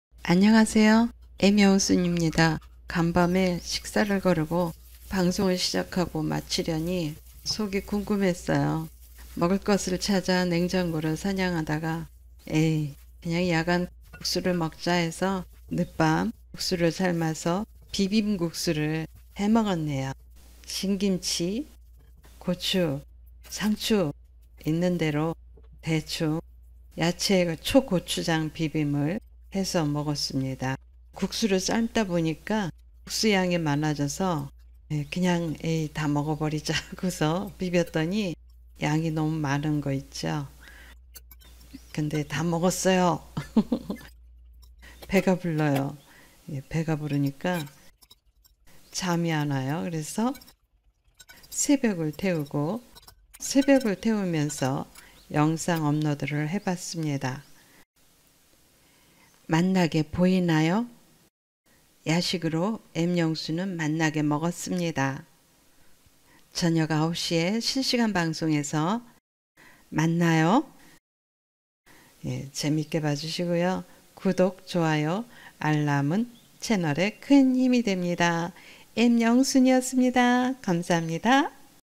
🌸​ 야간 야식 비빔국수ㅡ 빨리먹기 국수먹기 대충비빔국수 박말례할머니국수 영순tube
🌸​ 어제 저녁방송 전
거르고 야식이 생각나 뒤적 뒤적
하다가 .. 박말레 할머니 국수가 생각나서
국수를 삶았습니다.

야채 들 꺼내서 야채
를 해서 먹었습니다.

거북할줄 알았는데  속은 편하네요 .
대충국수 를 든든하게 먹고 나니 잘수가 없어서
간단하게 편집 하여 올려봅니다.
다음엔 잔치국수 를 야식으로 할까요? ㅎㅎ

채널에 큰 힘이 됩니다.
감사합니다 채널에 가입하여 혜택을 누려보세요.